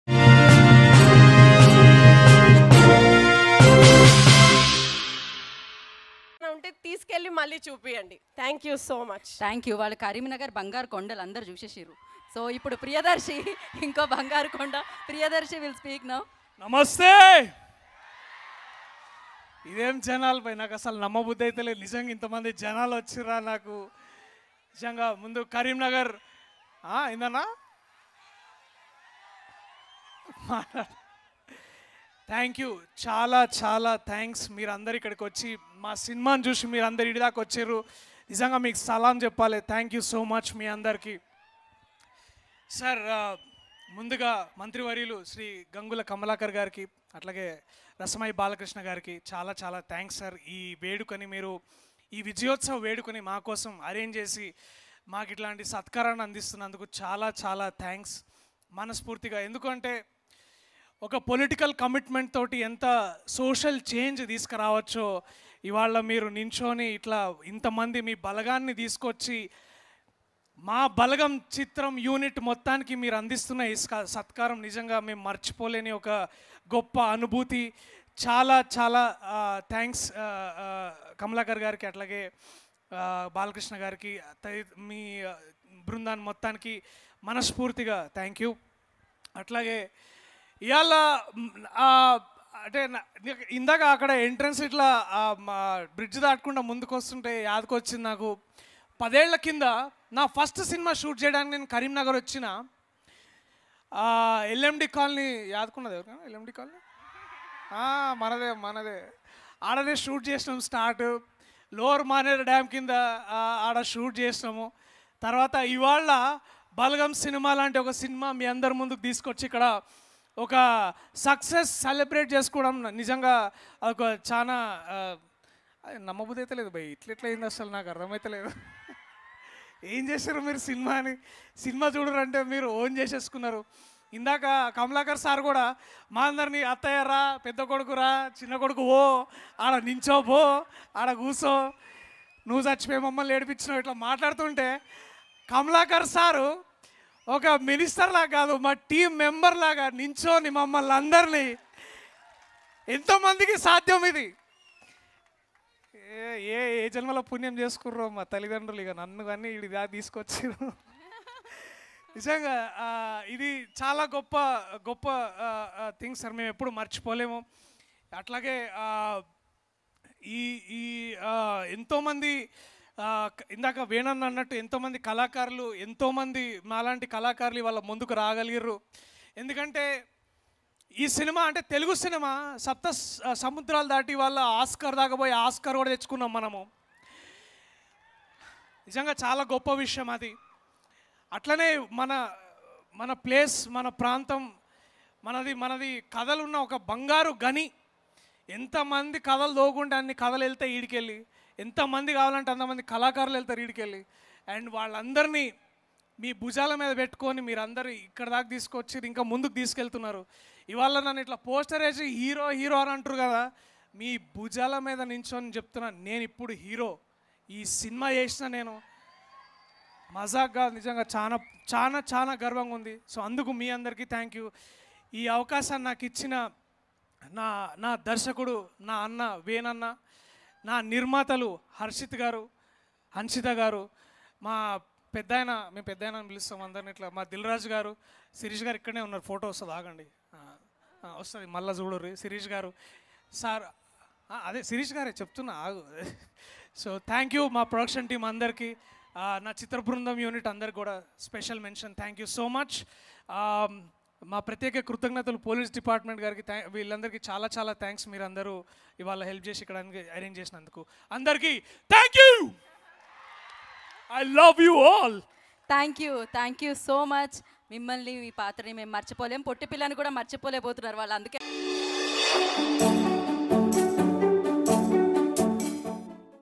Hero Priyadarshi Speech At Balagam Sucess Meet | Priyadarshi, Kavya Kalyanram | Socialpost TV
Movie Name: Balagam
Production House: Dil Raju Productions
Presented By: Shirish
Producers: Harshith Reddy, Hanshitha Reddy
Director: Venu Yeldandi
DOP: Acharya Venu
Editor: Madhu
PRO: Vamsi Kaka
Event Partner: Shreyas Media

Hello Friends,